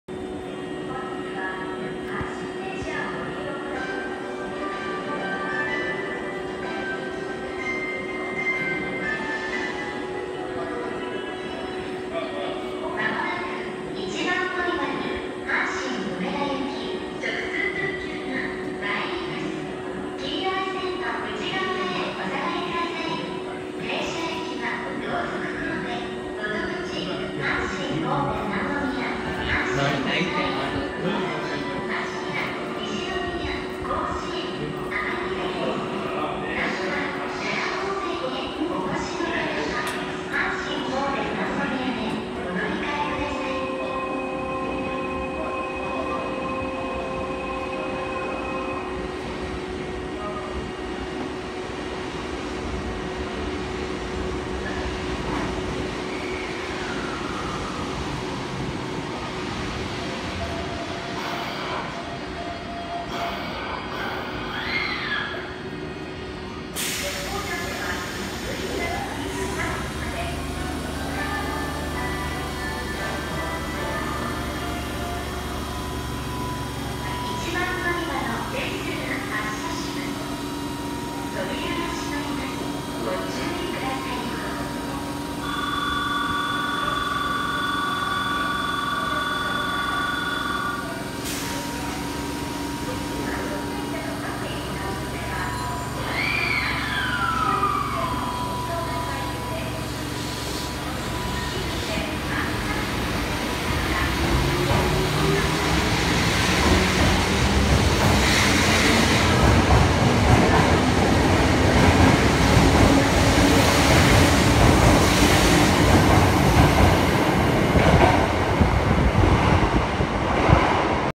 【山陽電気鉄道】神戸マラソンHM！ 5000系5016F 阪神梅田行き直通特急@新開地
阪急 阪神神戸高速線新開地駅にて。山陽姫路発阪神梅田行き直通特急 山陽電鉄5000系5016F 到着から発車までを撮影しました。
期間限定で神戸マラソンHMが掲出されていました。